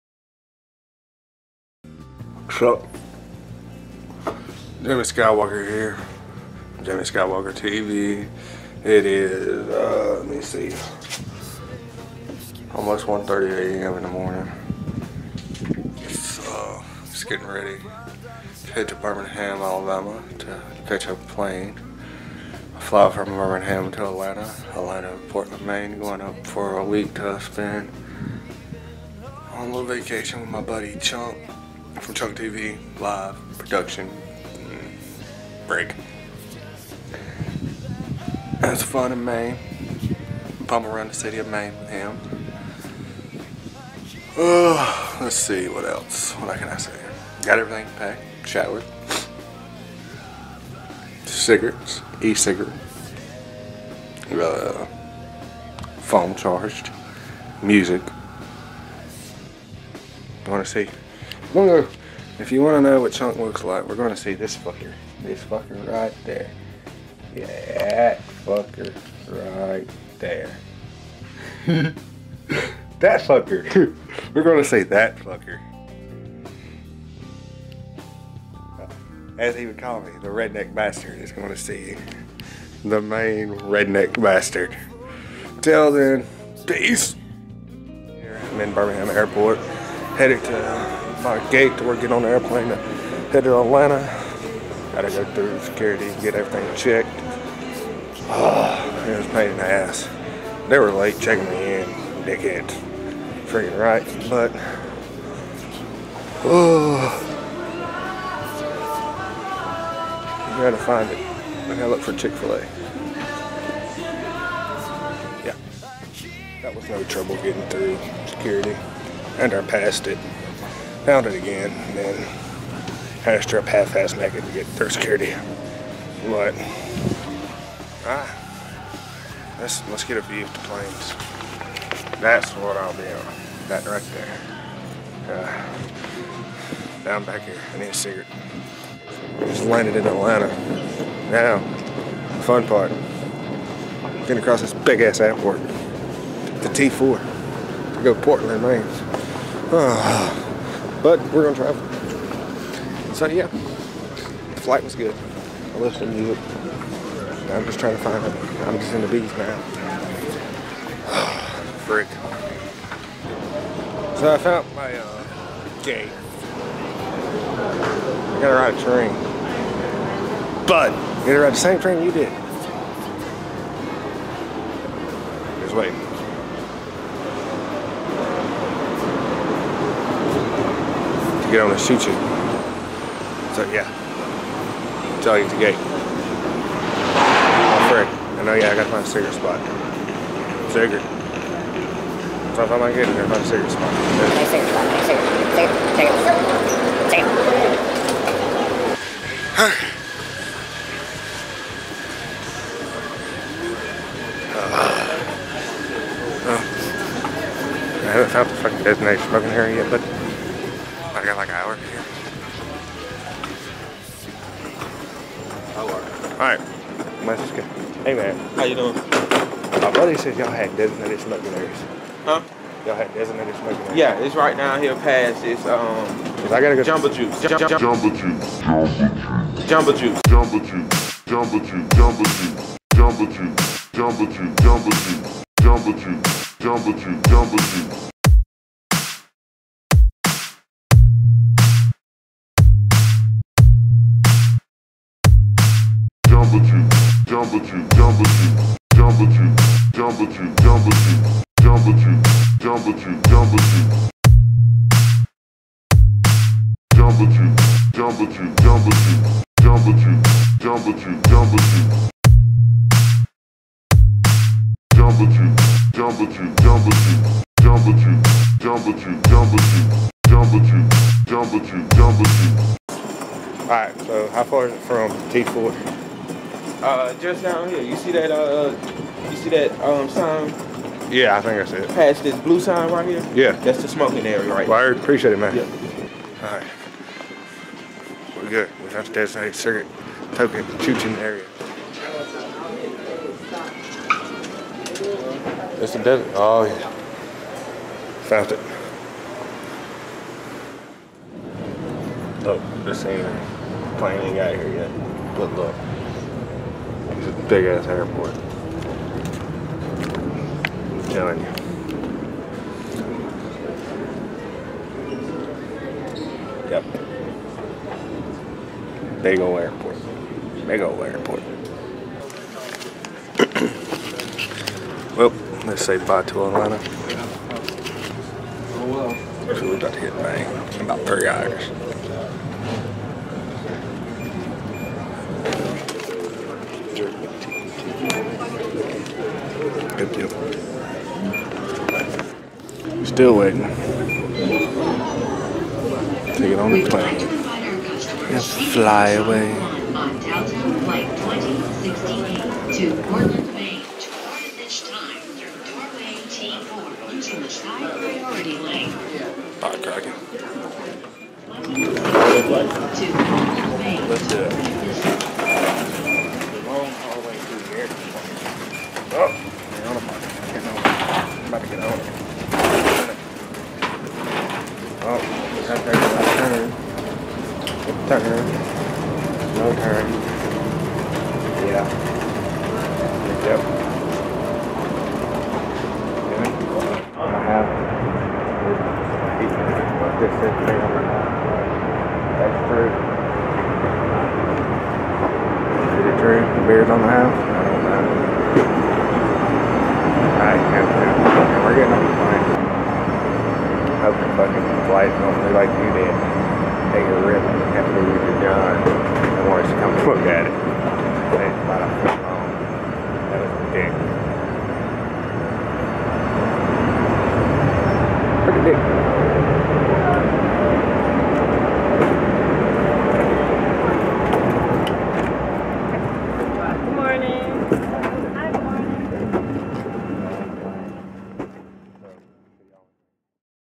RebelTv Goes to Maine (Part 1) - Jet Lag
Our first video shindig kicks off with a trip on up to Maine to visit a broadcaster and friend Chunk, from Chunk TV! Productions!